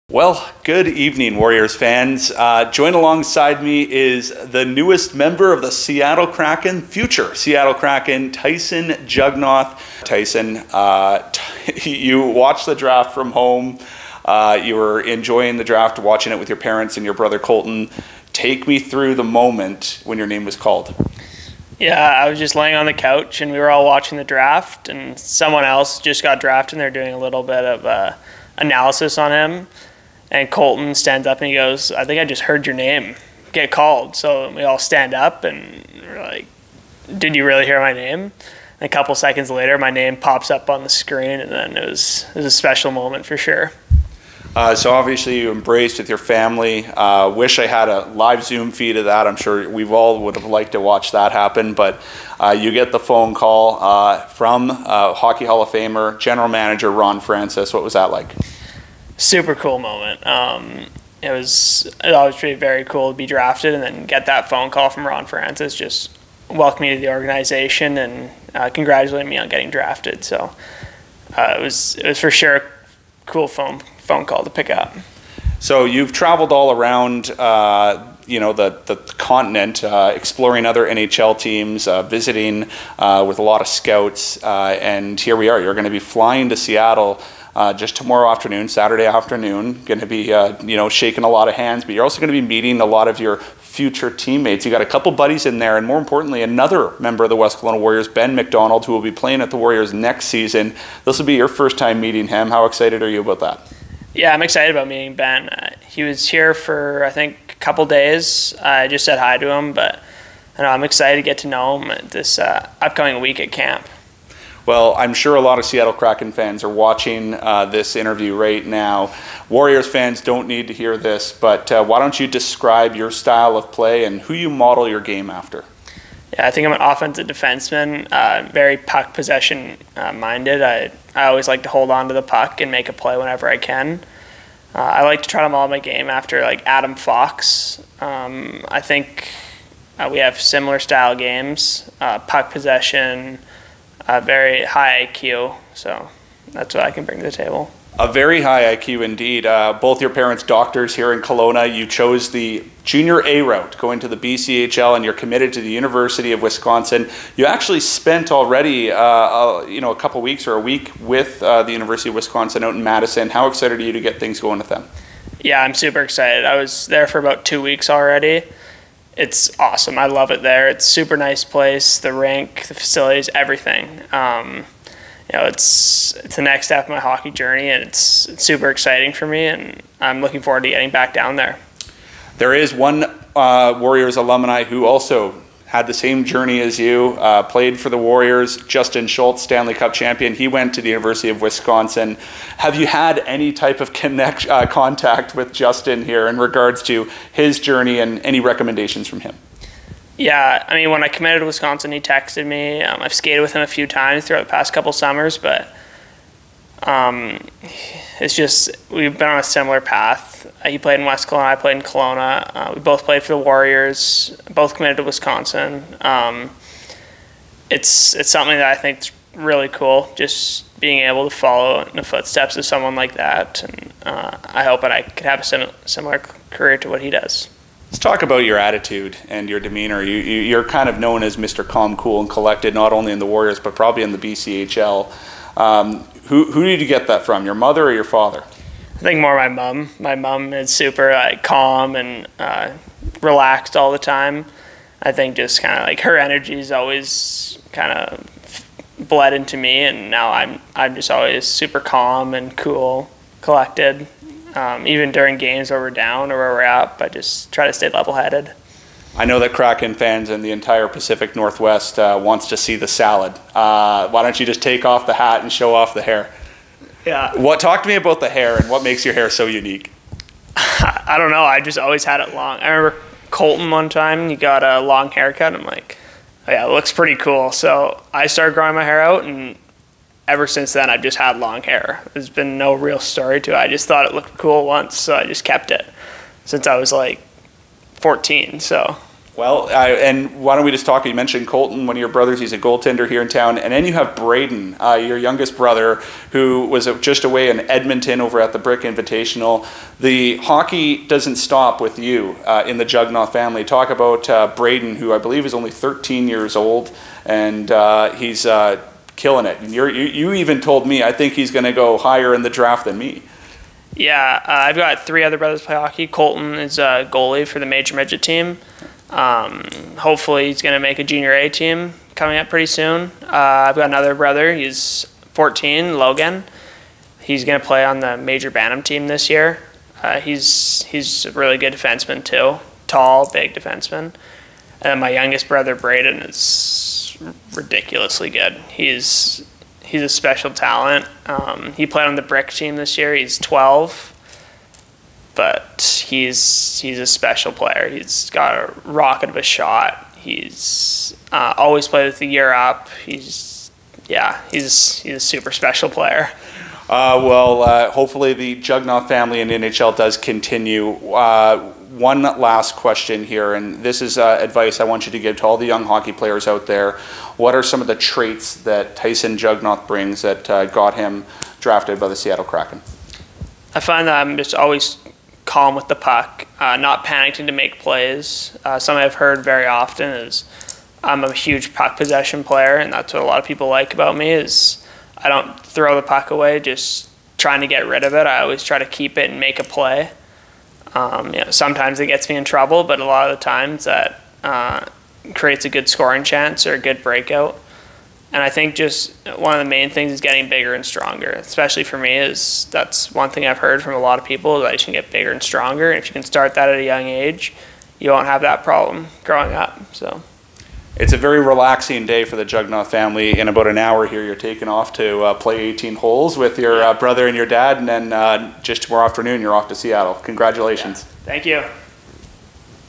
NHL Draft Recap: Tyson Jugnauth
Chase Johnston spends some time in the home of Tyson Jugnauth just a couple hours after he was selected by the Seattle Kraken 100th overall in the 4th round. July 8th, 2022 will be a memorable day in Warriors history with Ben MacDonald and Luke Devlin also being drafted. The Warriors led the BCHL in most players selected this year with this being the most successful draft year the Warriors have ever had.